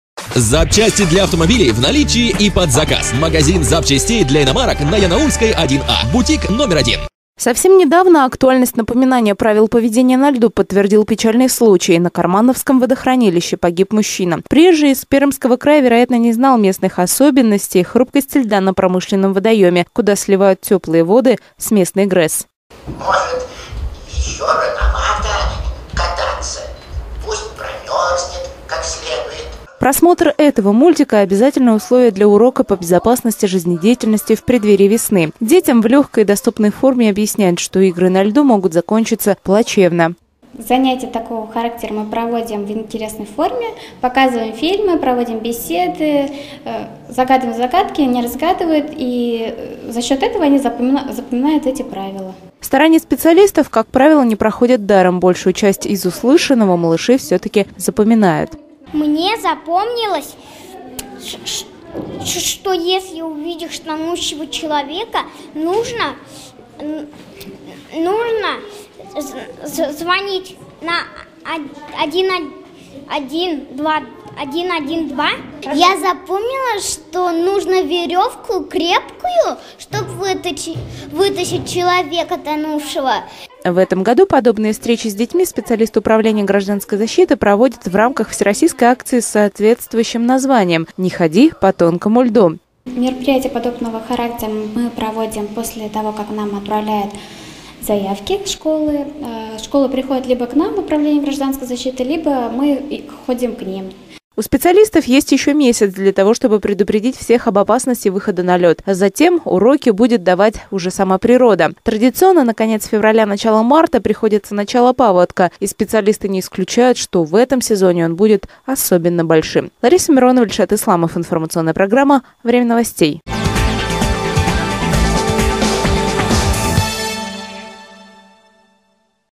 Не ходи по тонкому льду!
В преддверии весны  нефтекамцам напоминают о правилах поведения на льду. В конце февраля – начале марта поверхность рек и озер становиться особенно обманчивой. А мнимая крепость льда может подвести многих. Профилактические мероприятия начали проводить  не просто так.